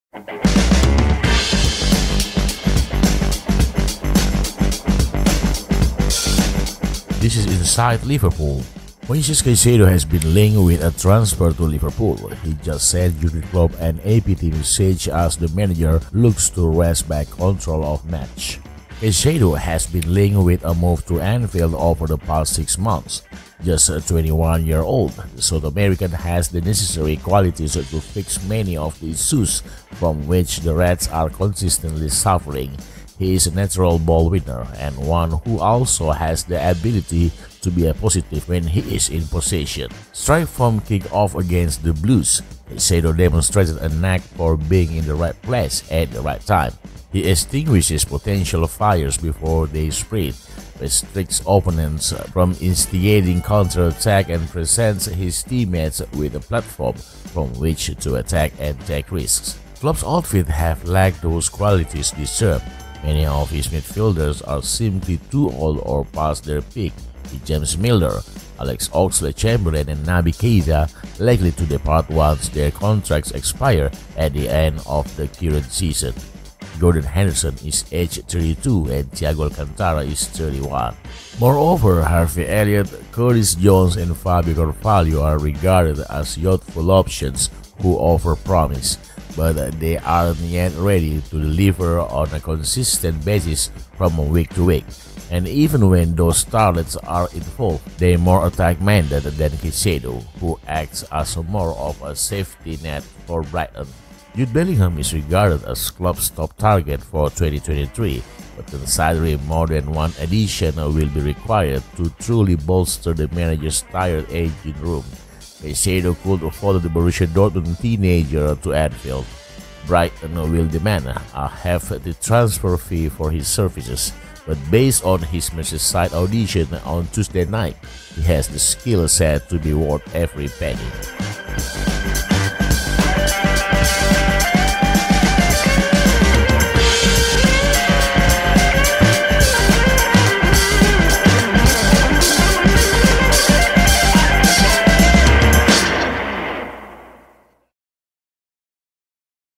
MOISÉS CAICEDO SENDS LIVERPOOL TRANSFER 'MESSAGE' AS IDEAL JÜRGEN KLOPP SOLUTION COMES TO TOWN
Moisés Caicedo has been linked with a transfer to Liverpool. He just sent Jürgen Klopp an apt message as the manager looks to wrest back control of matches.
Straight from kick-off against the Blues, Caicedo demonstrated a knack for being in the right place at the right time.
Klopp's outfit have lacked those qualities this term. Many of his midfielders are simply too old or past their peak.